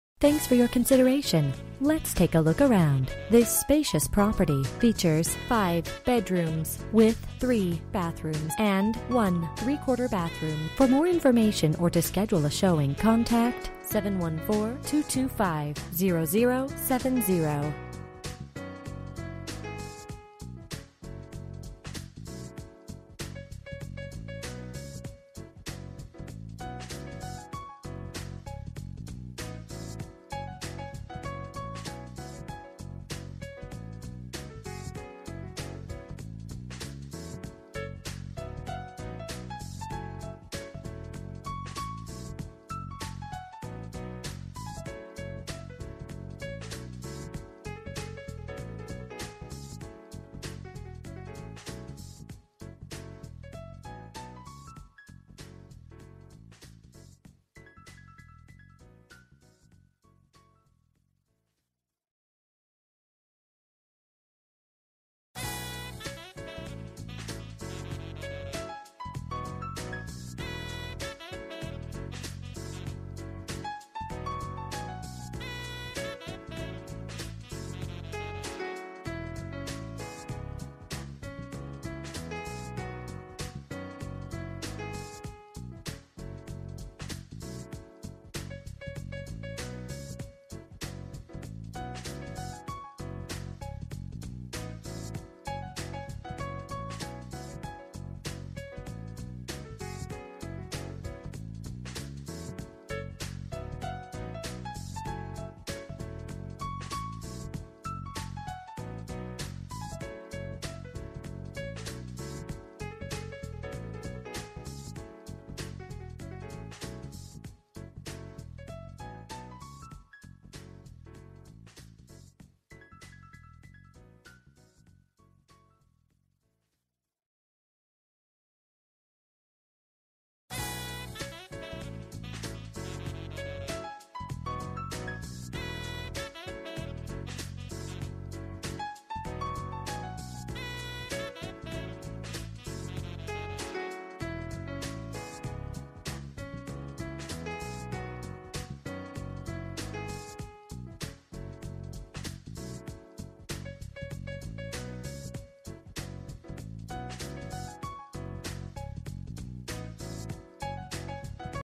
Property for sale - 2598 N Promontory Way, Orange, CA 92867
Immaculate home located in the prestigious and popular community of Serrano Heights, this impressive and stylish residence features 5 Bedrooms, (includes one bedroom with full bath downstairs) 4 Bathrooms and computer niche. Other highlights include new wood flooring installed upstairs , recessed lighting and new interior paint. Fabulous chef's delight kitchen host granite counters, center island with sink, double electric oven, built-in microwave, new stainless dishwasher, new garbage disposal, walk-in pantry and breakfast nook. Adjacent to the kitchen is the spacious family room with entertainment center and warm fireplace. A Formal living and dining room to entertain your guest. Spacious Master suite with retreat and cozy fireplace. Beautiful master bath with dual sinks, vanity, bath tub, walk-in shower and generous walk-in closet. Spacious backyard. Three car garage with direct access. An upstairs laundry room for your convenience. Near hiking/walking trails, parks, shopping and Anaheim hills school. Ready to move-in. Your clients will love this home!

Bedrooms: 5
Bathrooms: 3 full and 1 three-quarter Bathrooms
Square Feet: 0
Price: $4,300

MLS ID: PW17100971